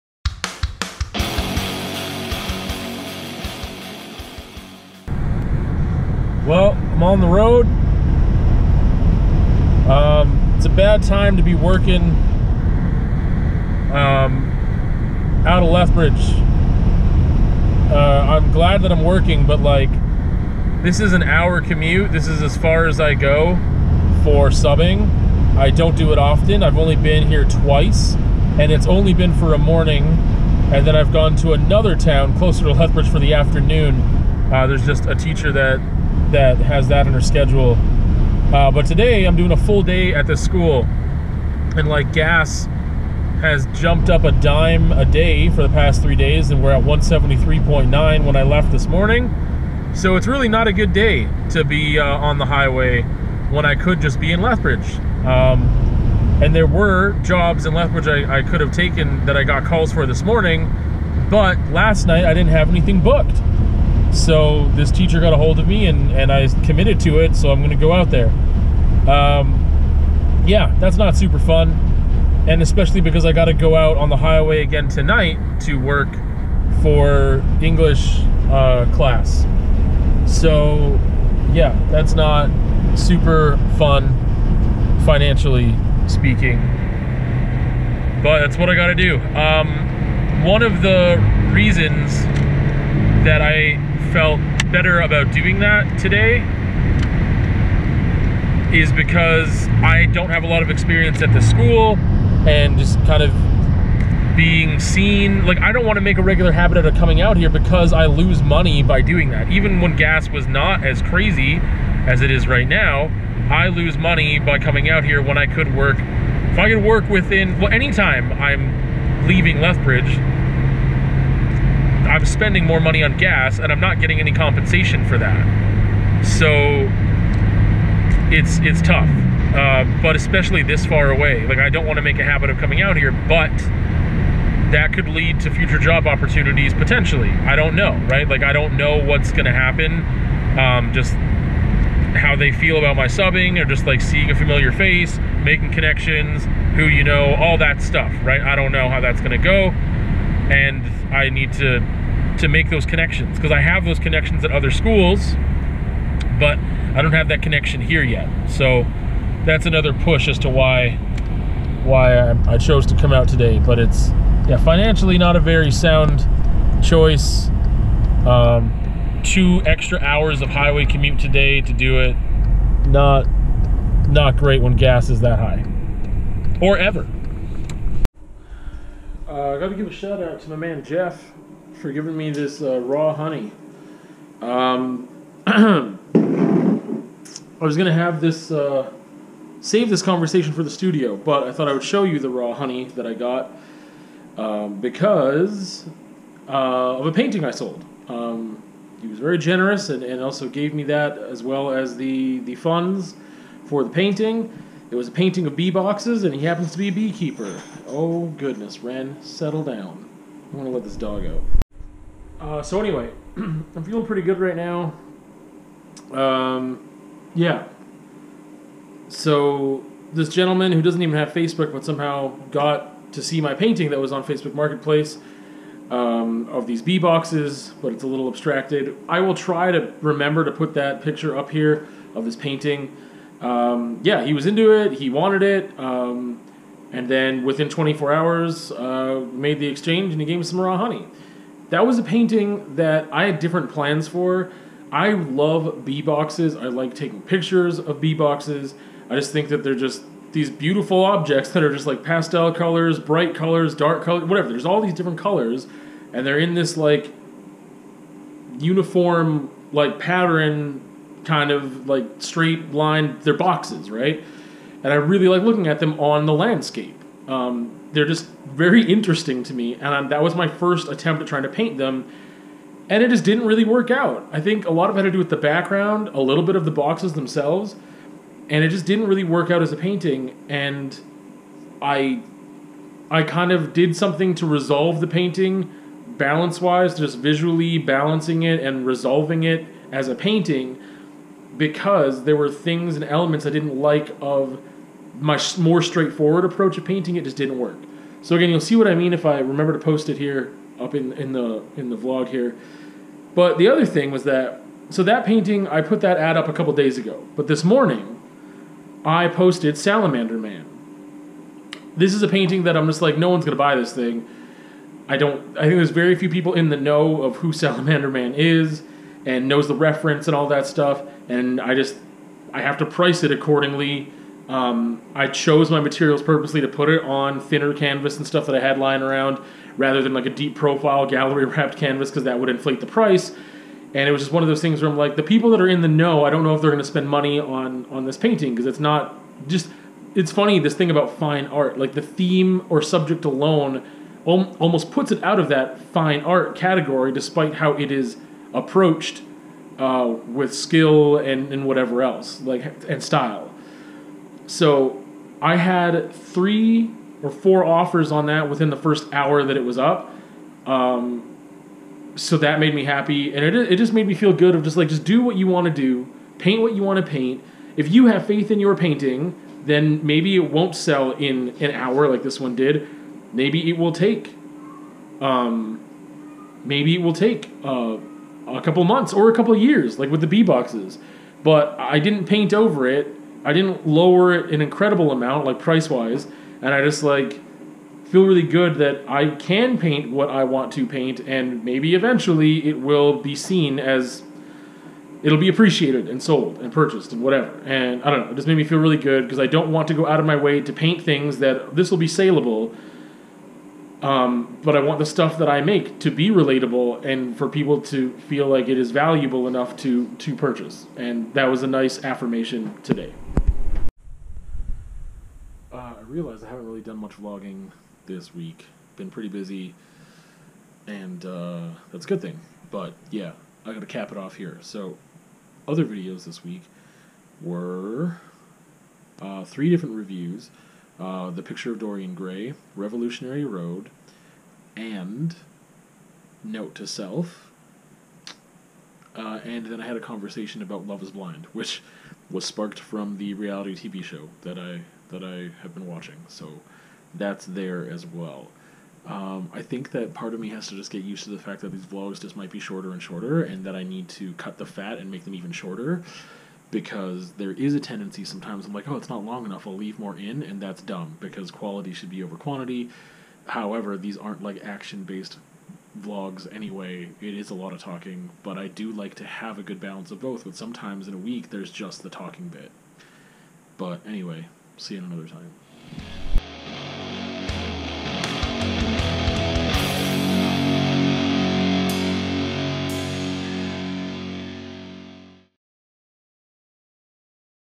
Gas is bananas/Art what you Believe in! (VLOG)
I have a long commute to work in the midst of gas prices being crazy high, I also sell a couple pieces and feel excited about continuing to make art I believe in. (VLOG - WEEK 10 YEAR 4)

Artist Life, Alberta, Vlogger, Vlog, Vlogging.